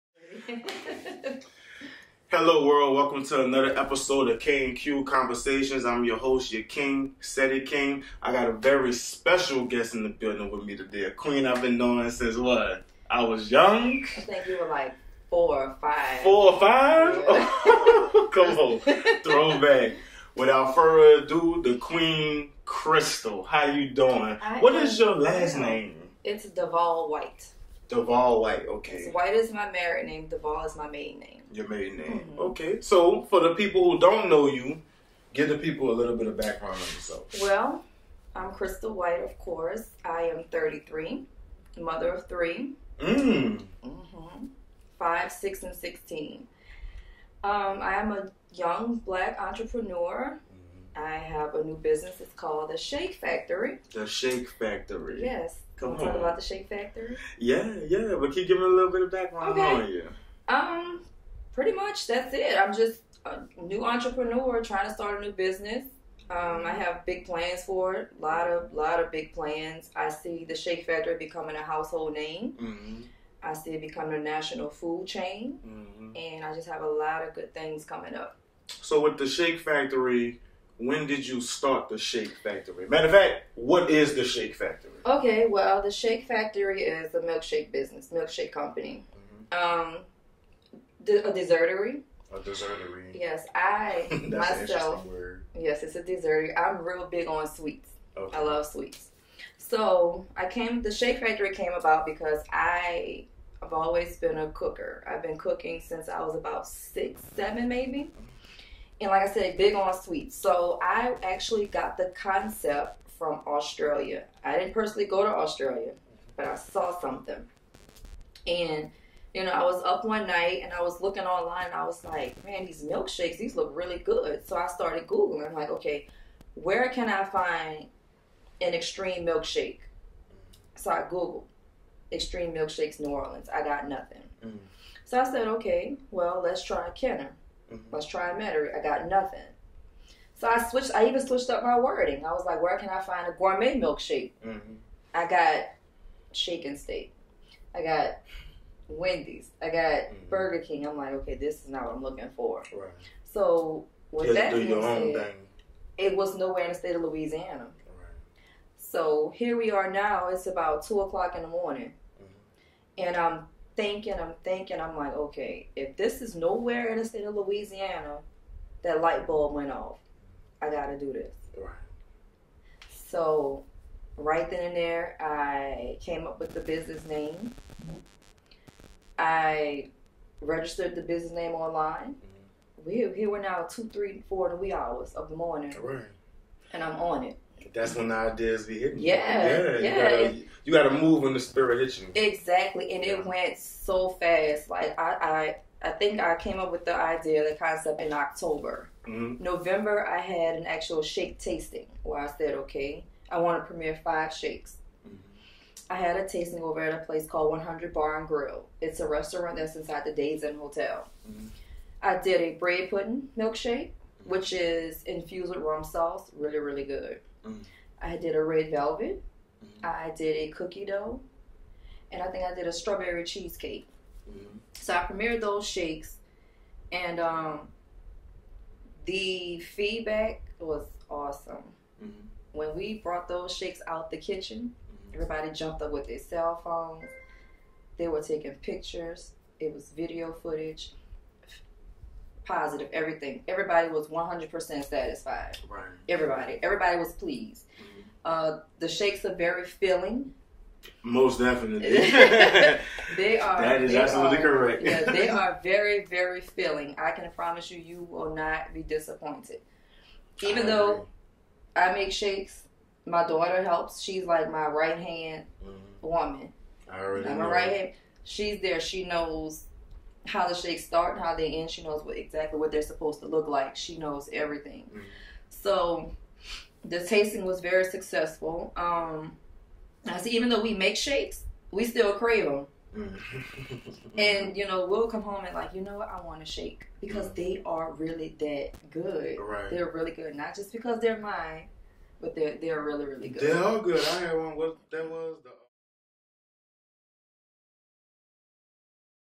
K&Q Konversation - Crystal White Part 1
Konversation with Ms. Crystal White of The Shake Factory.